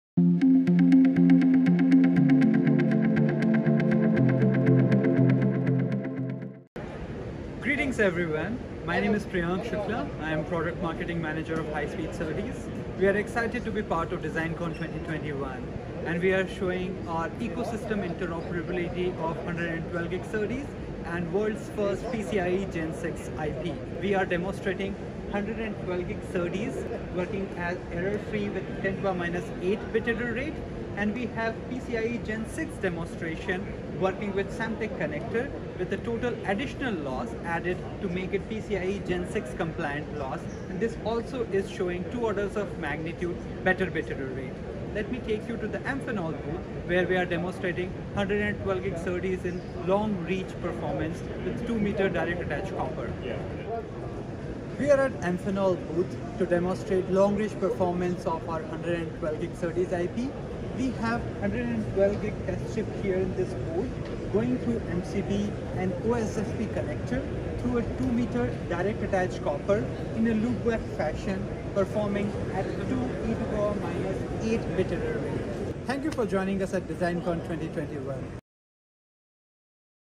DesignCon 2021 112G Ethernet & PCIe 6.0 IP Demos -- Synopsys
This video features Synopsys' silicon-proven DesignWare 112G Ethernet and PCIe 6.0 PHY IP solutions successfully interoperating with Samtec's AI/ML edge connectors and Amphenol's Direct Attach Copper (DAC) cables with superior Bit Error Rates (BERs) at maximum performance.